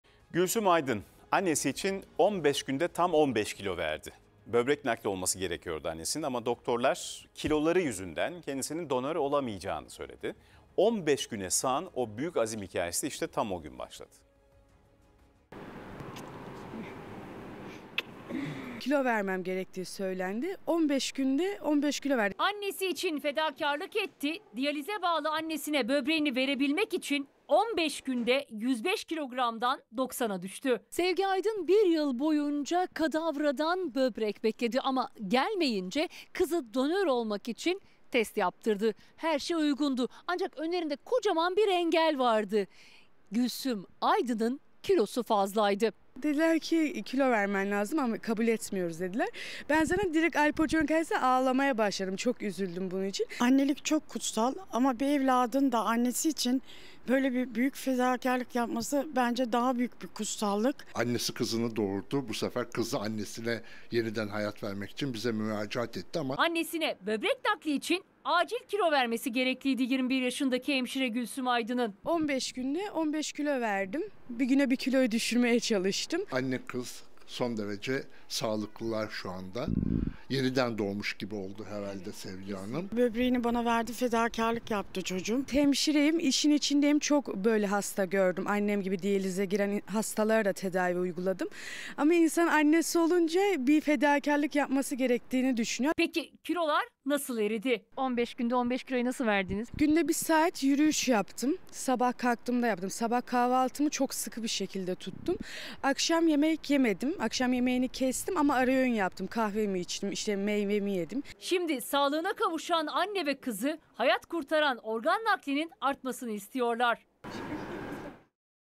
Annesi için 15 kilo verdi - atv Haber 7 Şubat 2022
Diyalize bağlı annesine böbreğini verebilmek için 15 günde 15 kilo verdi.